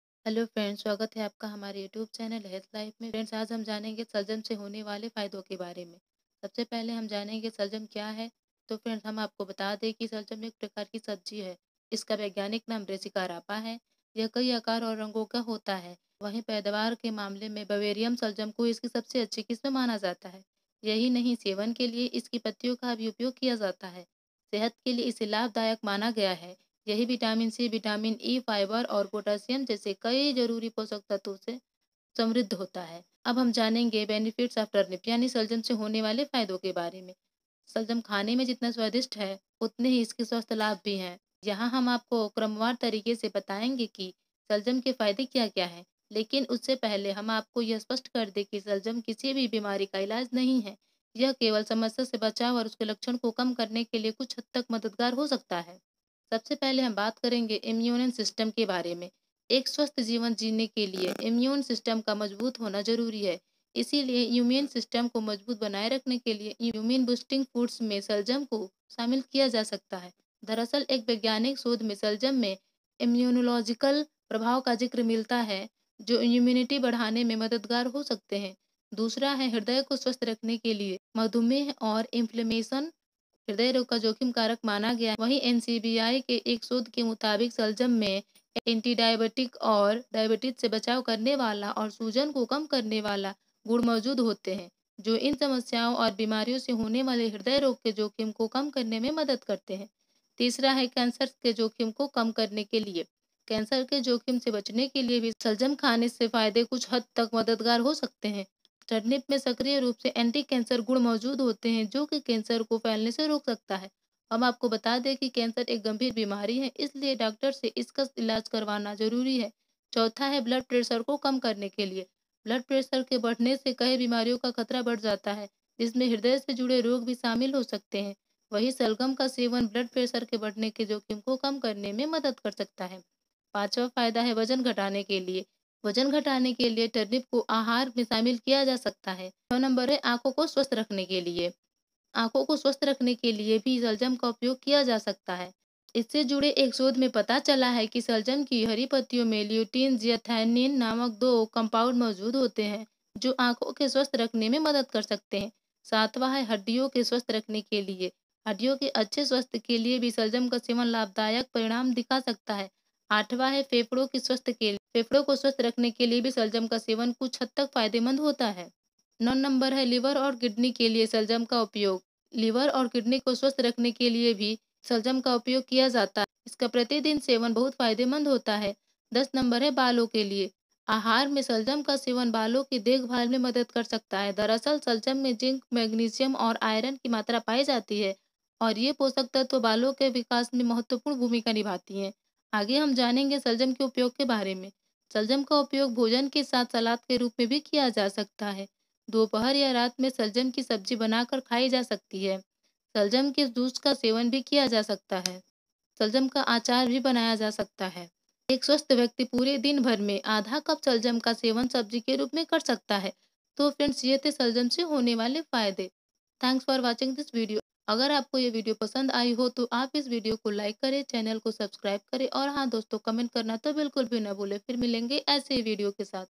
Health benefits of turnip. Salgam खाने से होने वाले बेहतरीन फायदे।
Salgam का सेवन हमारे शरीर के लिऐ लाभदायक होता है। और यह कई प्रकार का होता है।यह अनेक तरीको से हमारे शरीर के लिए लाभदायक होता है। इसका सेवन ब्लड प्रेसर को भी कंट्रोल करता है। इसके सेवन से हृदय रोग का खतरा कम होता है।और यह अनेक प्रकार से हमारे शरीर के लिऐ लाभदायक होता है।